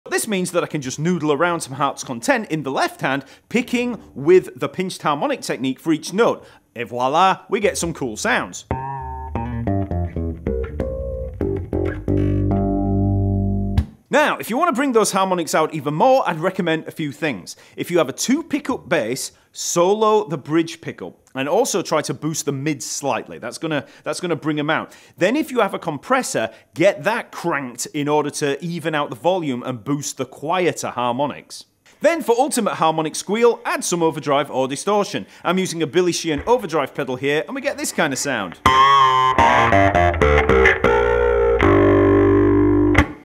Try This Technique To Sound EPIC!
In my latest video, we'll be uncovering the key to perfect "pinched harmonics" on bass and how to make them sound EPIC!